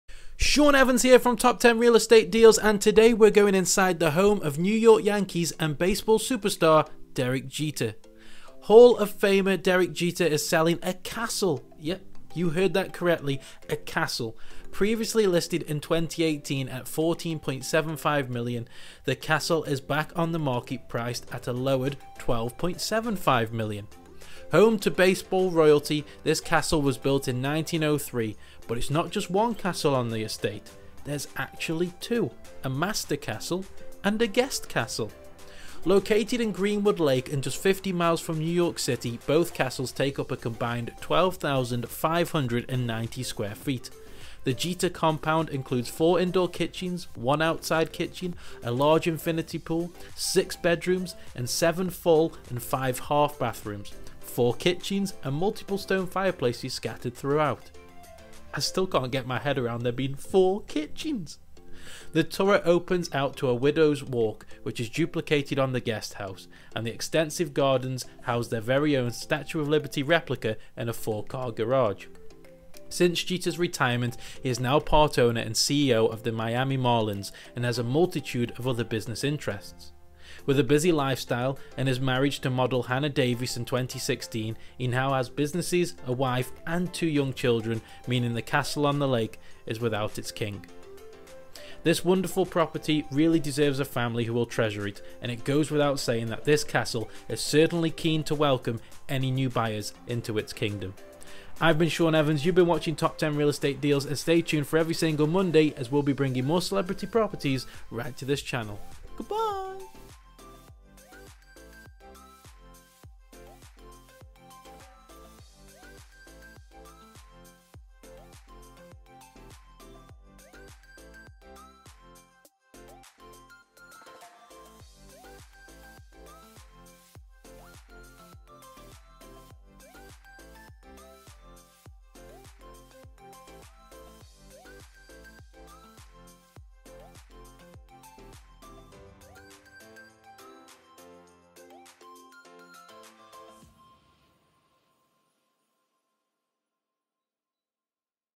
Inside Derek Jeter's $12.75 Million Castle
Take a look inside this Greenwood Lake castle owned by Derek Jeter that is currently on the market for $12.75 million dollars.

Derick Jeter Photo Credit - Ovidiu Hrubaru / Shutterstock.com
Property Photo Credit - Wright Bros.